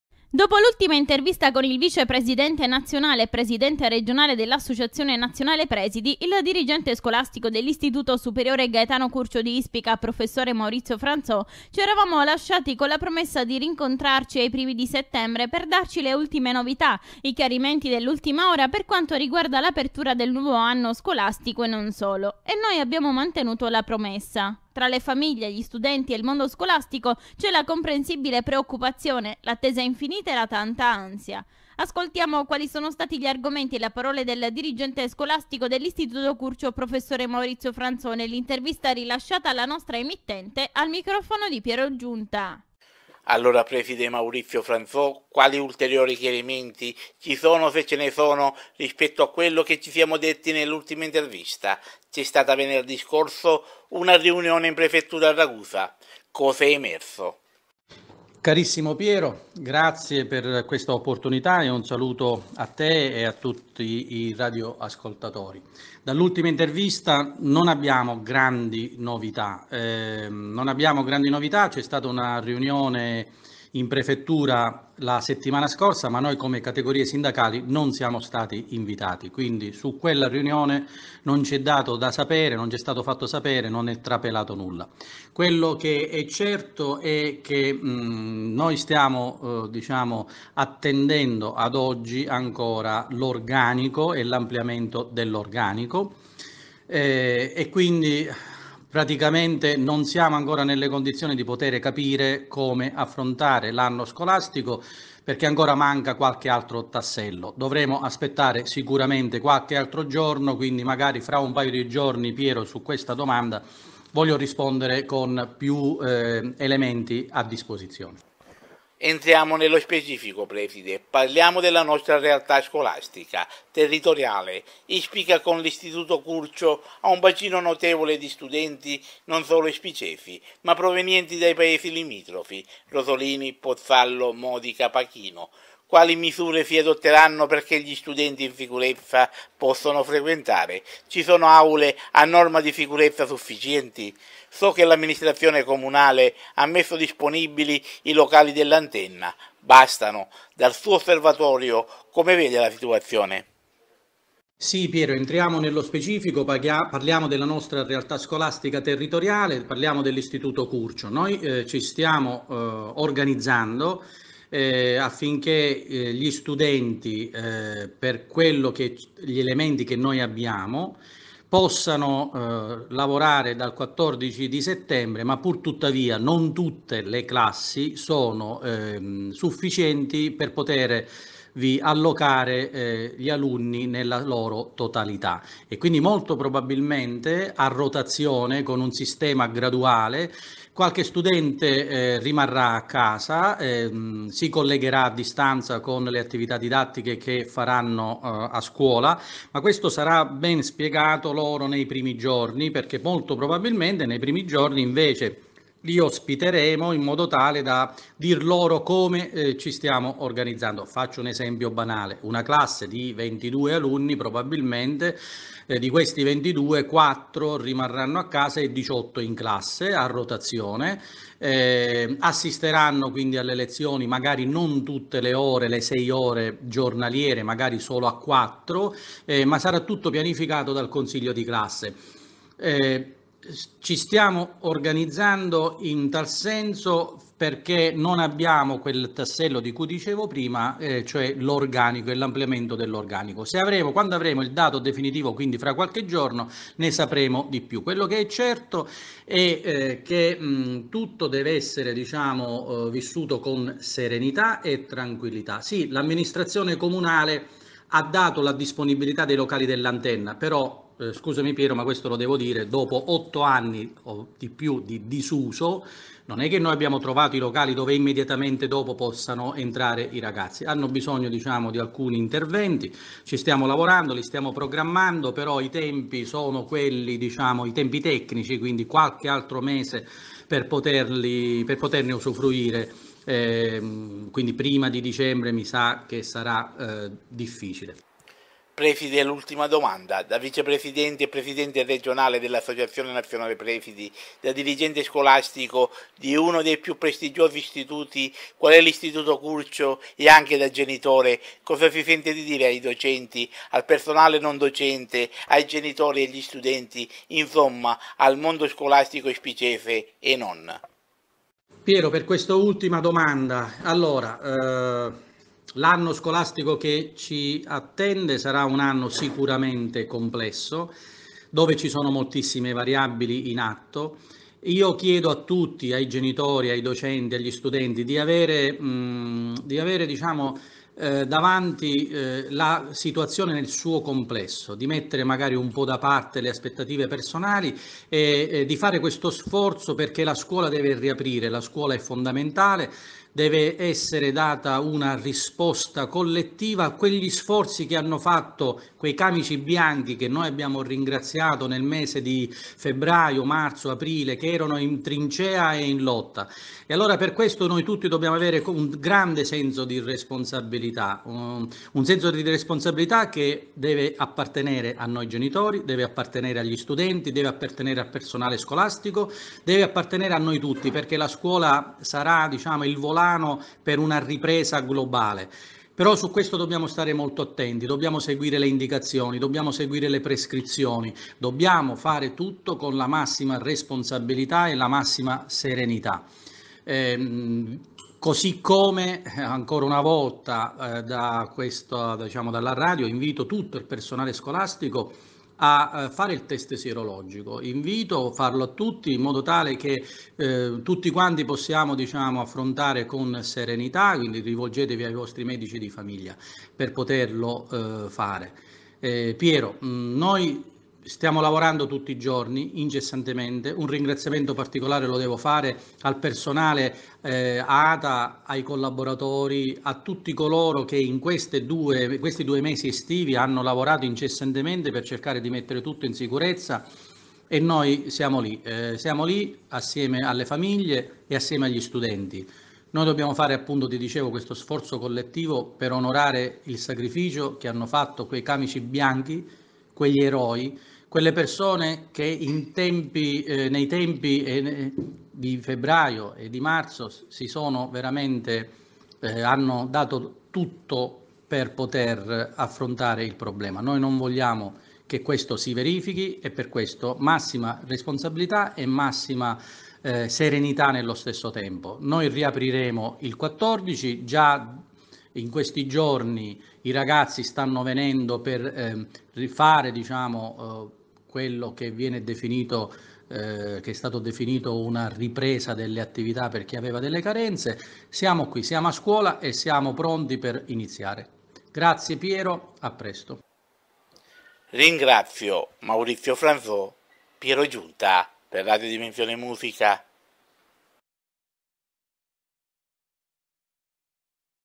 QUATTRO CHIACCHIERE RADIOFONICHE - INTERVISTA A MAURIZIO FRANZO'
Quattro chiacchiere ed un caffè con: Maurizio Franzò
Vicepresidente nazionale e Presidente Regionale ANP - Dirigente Scolastico Istituto Gaetano Curcio di Ispica.

L'intervista del giorno:
di Piero Giunta
Dopo l’ultima intervista con il Vicepresidente Nazionale e Presidente Regionale dell’Associazione Nazionale Presidi, il Dirigente Scolastico dell’Istituto Superiore Gaetano Curcio di Ispica Professor Maurizio Franzò, c’eravamo lasciati con la promessa di rincontrarci ai primi di settembre per darci le ultime novità, i chiarimenti dell’ultima ora per quanto riguarda l’apertura del nuovo anno scolastico e non solo. E noi abbiamo mantenuto la promessa. Tra le famiglie, gli studenti e il mondo scolastico c’è la comprensibile preoccupazione, l’attesa infinita e la tanta ansia. Ascoltiamo quali sono stati gli argomenti e le parole del Dirigente Scolastico dell’Istituto Curcio Prof. Maurizio Franzò nell’intervista rilasciata alla nostra emittente al microfono di Piero Giunta.